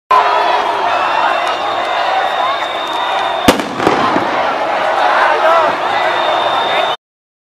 Manifestação e Protesto Acorda Brasil em Brasília/DF - Parte 13
Manifestação e Protesto Acorda Brasil em Brasília/DF - 60 mil pessoas aprox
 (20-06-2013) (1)